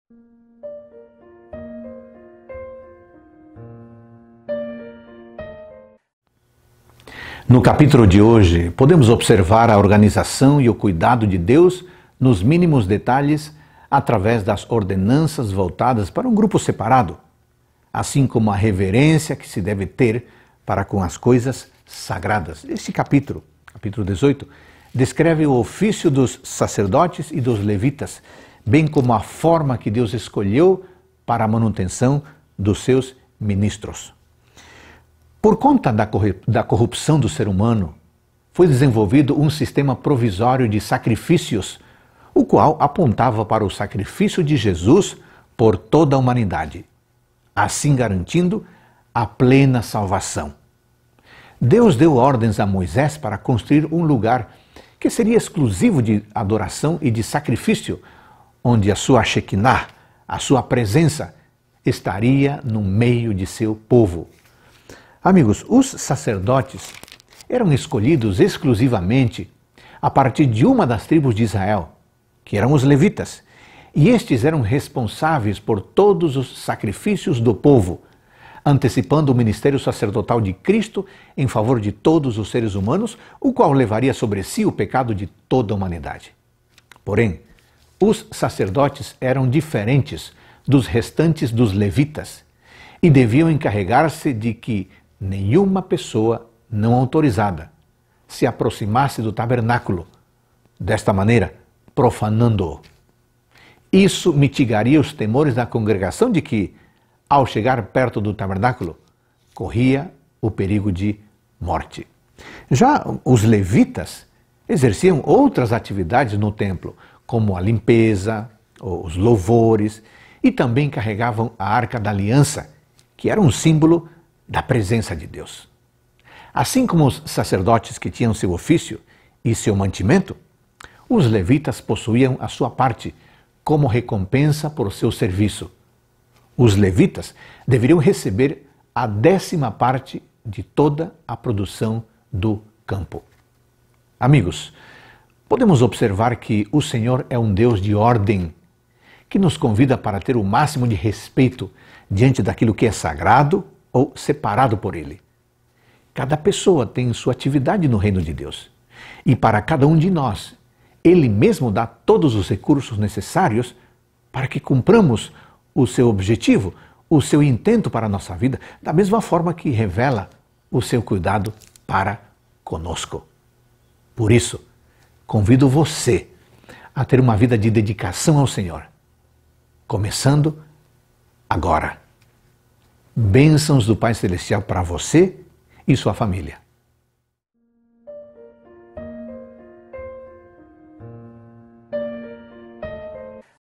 Números 18 - Reavivados por Sua Palavra
As informações aqui registradas sobre os deveres dos sacerdotes e dos levitas não são novas. Repete-se nesta ocasião particular para enfatizar o princípio que havia sido demonstrado tão dramaticamente: as coisas sagradas não podem ser profanadas.